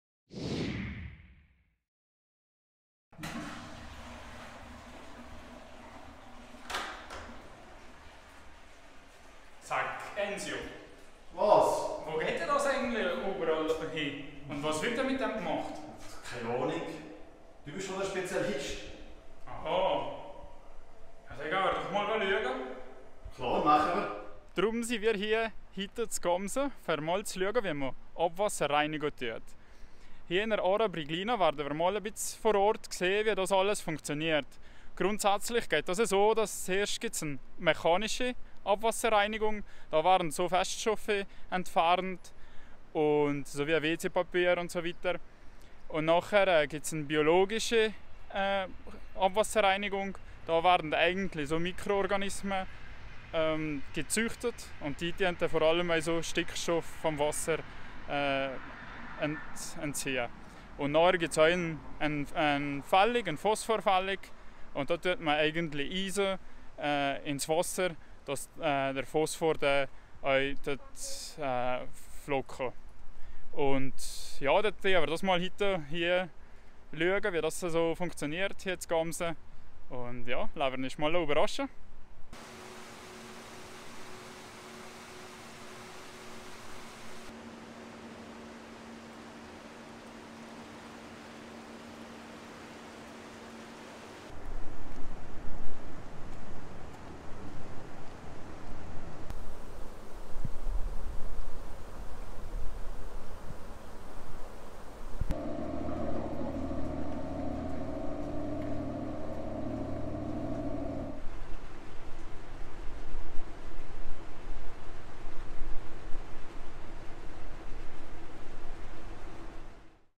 Besuch der ARA
Was passiert mit unserem Abwasser? Wir haben uns auf den Weg gemacht und waren in der ARA von BriGliNa zu Besuch und haben uns vor Ort erkundet.